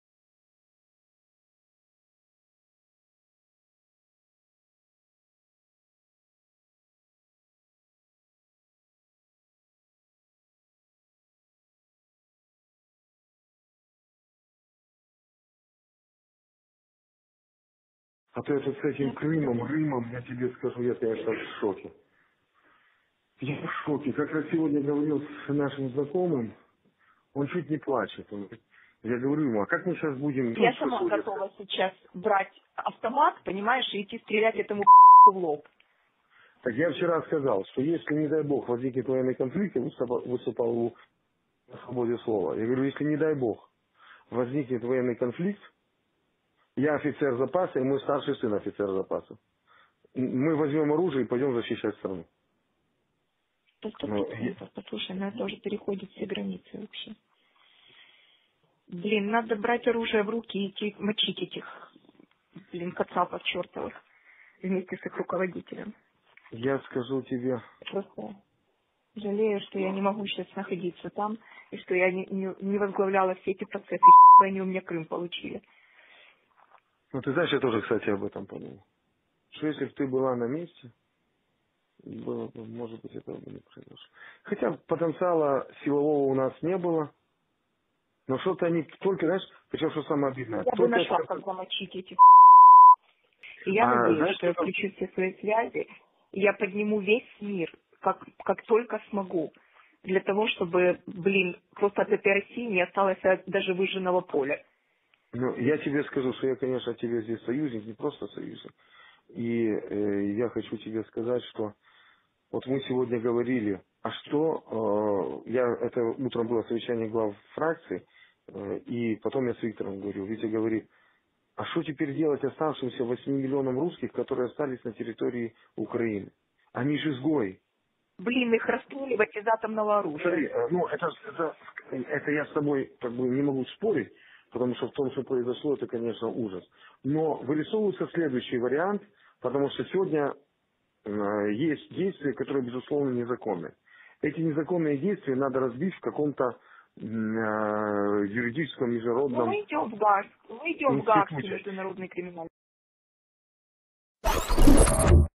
Former Ukraine Prime Minister Tymoshenko; "Kill 8 Million Russians with Nukes leaked phone Call / Eh
Former Ukraine Prime Minister Tymoshenko; "Kill 8 Million Russians with Nukes leaked phone Call / Ehemaligen Ministerpräsidentin Timoschenko Ukraine; "Tötet 8 Millionen Russen mit Nukes durchgesickerte phone Anrufen

More Clips how the Ukraine try to stop the War in Peace and real picture of what going on there please watch